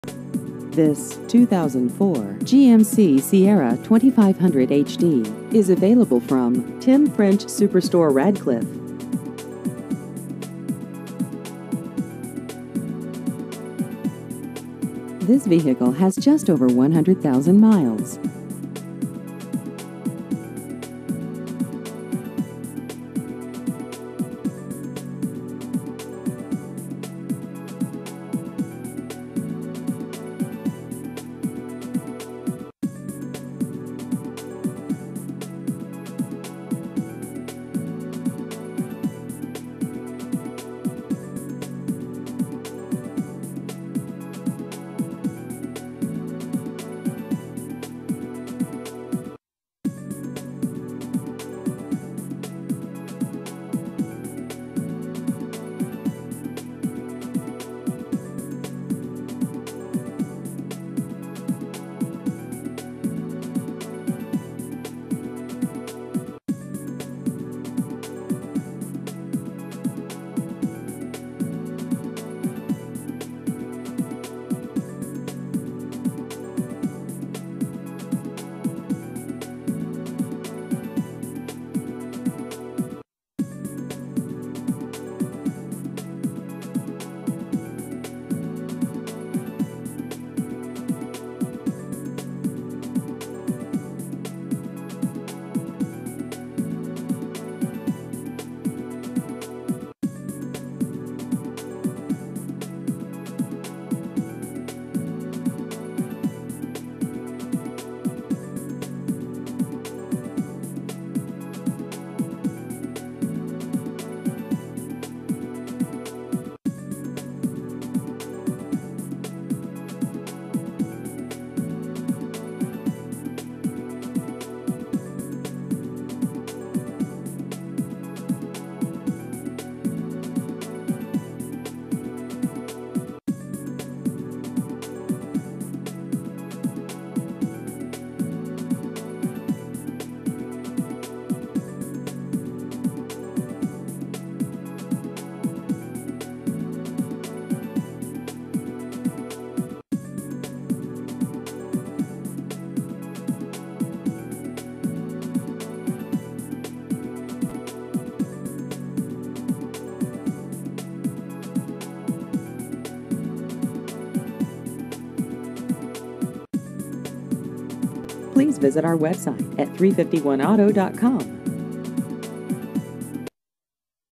2004 GMC Sierra 2500HD Used Cars Radcliff KY
This 2004 GMC Sierra 2500HD is available from Tim French Superstore Radcliff.